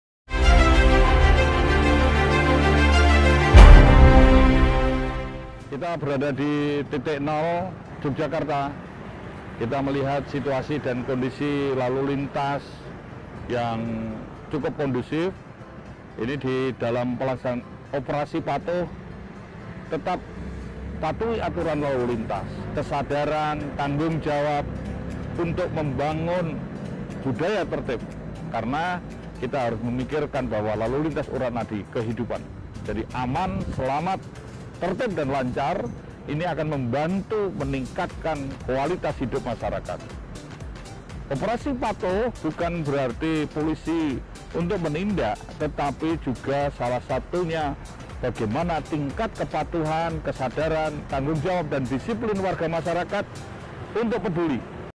OPERASI PATUH 2020 (Bangun Kesadaran dan Tanggung Jawab Berlalu Lintas)
Bangun Kesadaran dan Tanggung Jawab akan Keselamatan dan Keamanan demi Kelancaran Lalu Lintas
STOP PELANGGARAN
STOP KECELAKAAN
KESELAMATAN UNTUK KEMANUSIAAN